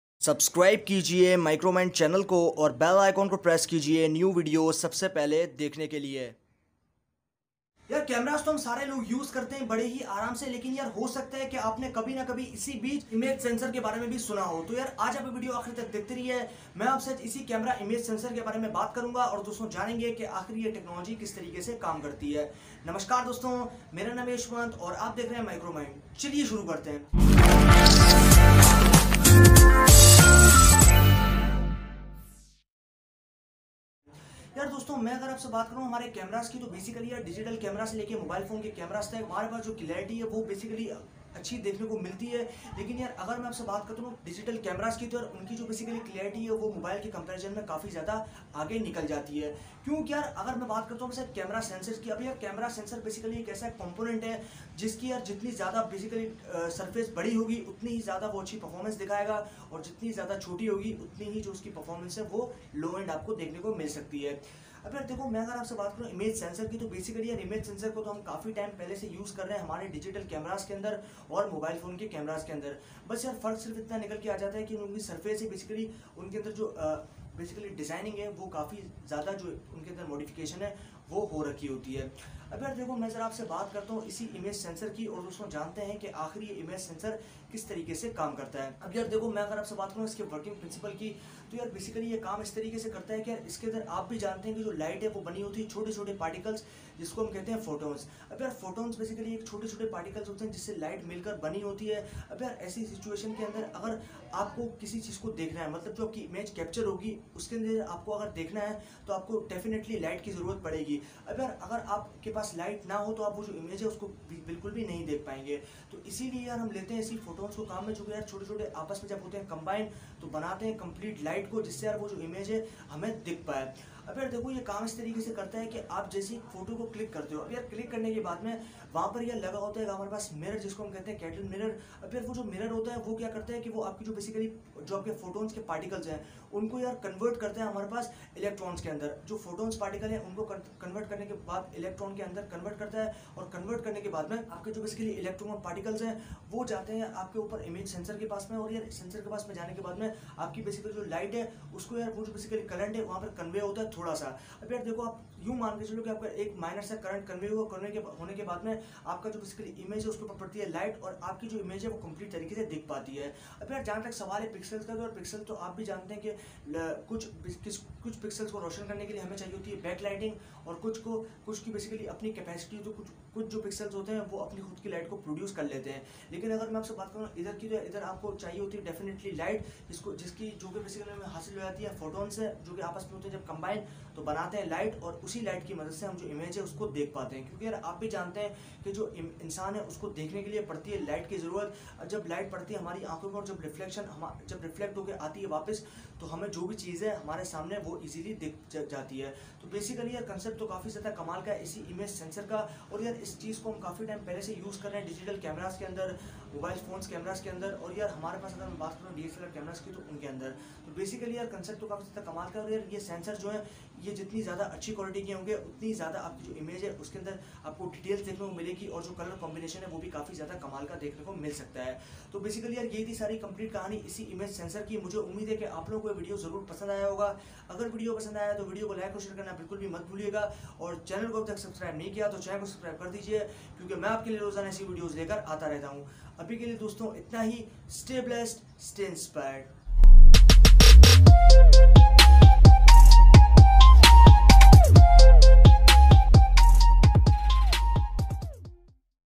Camera Sensor Explained? Captured Something?
Hello Friends, In this video I'm going to explain you a complete concept of image sensor.
An image sensor or imager is a sensor that detects and conveys information used to make an image. It does so by converting the variable attenuation of light waves (as they pass through or reflect off objects) into signals, small bursts of current that convey the information.